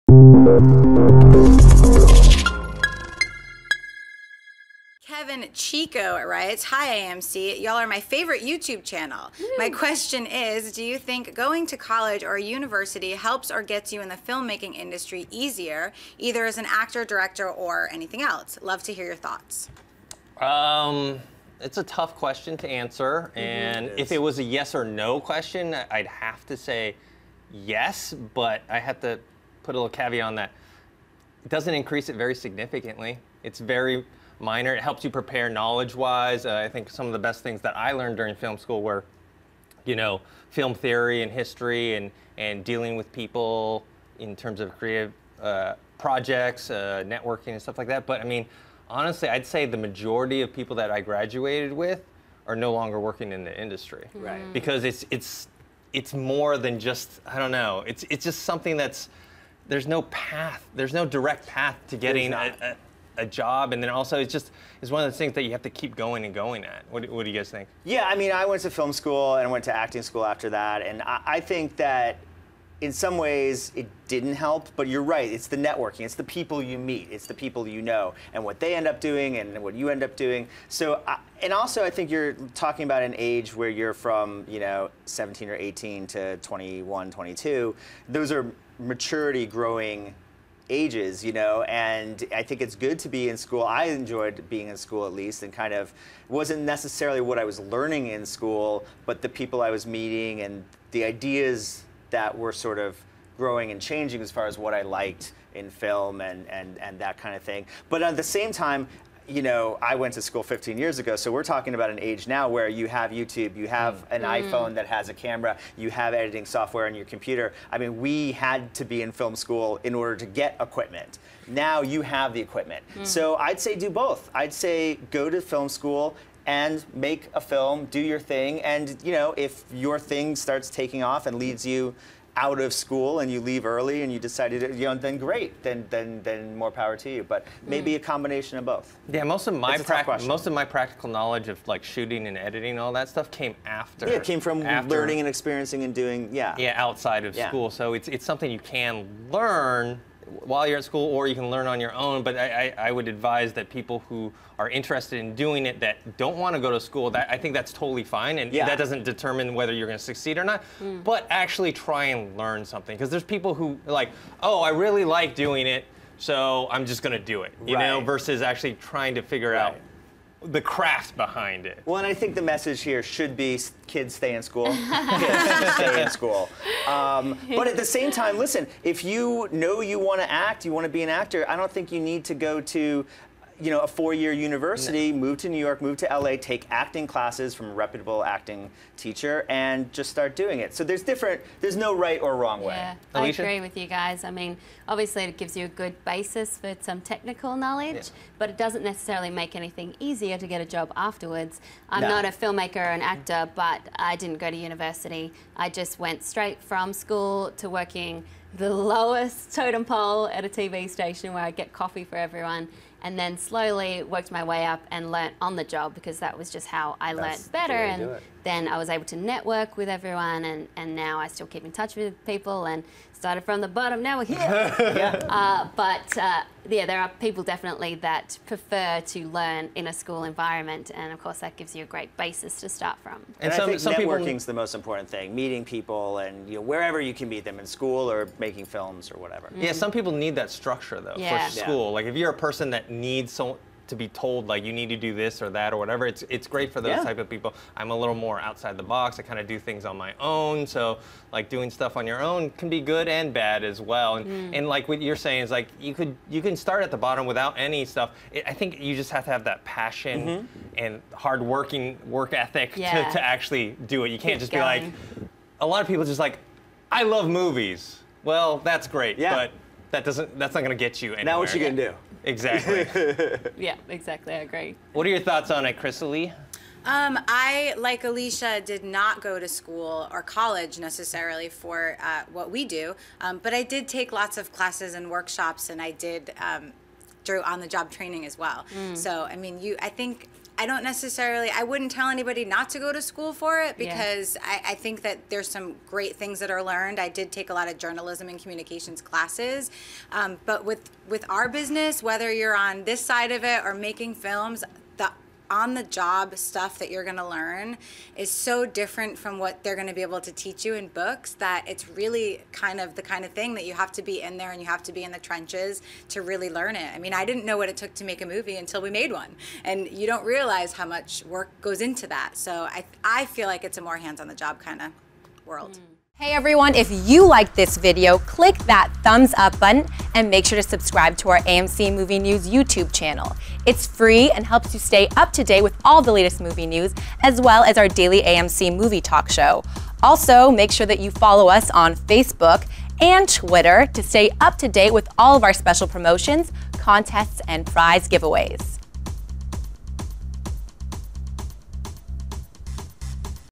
Does Going To College Help You To Break Into The Film Industry - AMC Movie News
Kevin Chico writes: hi amc y'all are my favorite youtube channel, my question is do you think going to college or a university  helps or gets you in the film making industry easier, either  as a actor, director, etc. Love to hear your thoughts